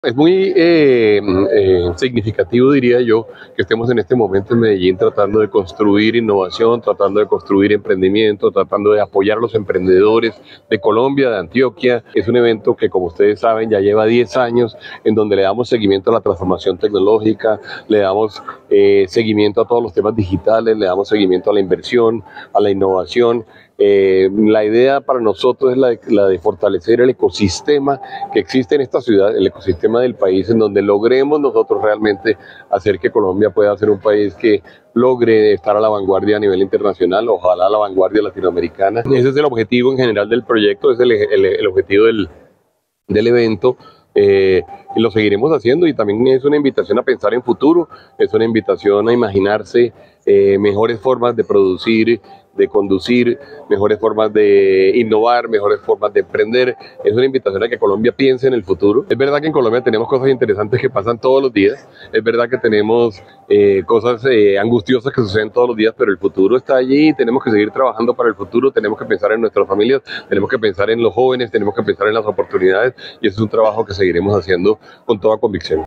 Bruce Mac Master, presidente de la ANDI en Innovation Land Summit 2024
🔴📣 Bruce Mac Master, presidente nacional de la ANDI, hizo un vehemente llamado para dejar de lado las diferencias políticas e ideológicas y concentrarnos en trabajar por el país. Además, invitó a las empresas para que no dejen de invertir y piensen en el futuro.

Esto en el marco de la décima edición de Innovation Land Summit en Medellín.

🔴 Además, cuando se refirió a la situación de Sura en el país, indicó que, "es una gran tristeza la salida de la EPS Sura".